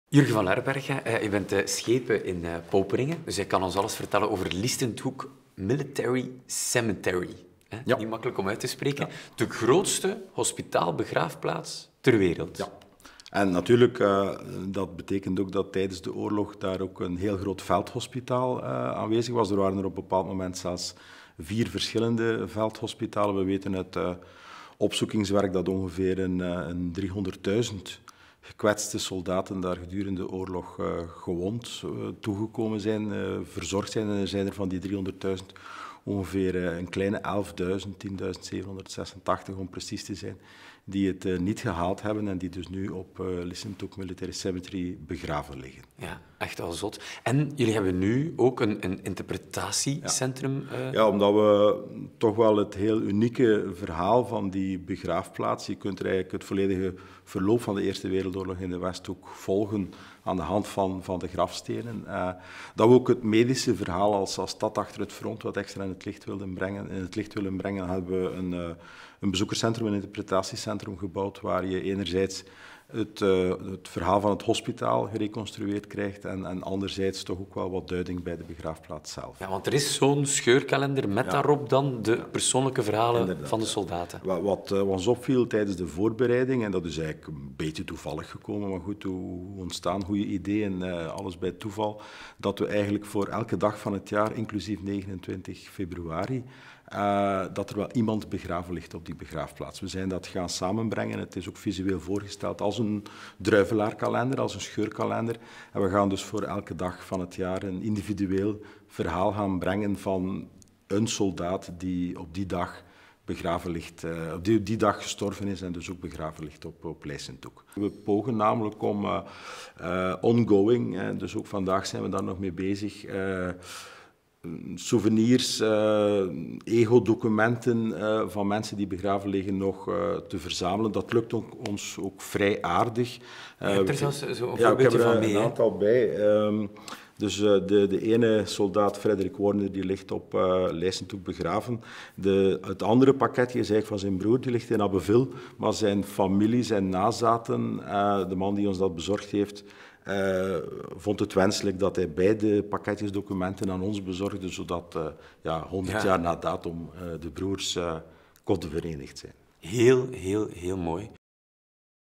Erfgoedprijs 2015 - genomineerde Lijssenthoek Military Cemetery Poperinge
Om de twee jaar reikt de Provincie West-Vlaanderen een prijs uit voor erfgoed.
In dit filmpje hoor je Jurgen Vanlerberghe van stad Poperinge, die genomineerd was voor Lijssenthoek Military Cemetery en uiteindelijk ook in de prijzen viel.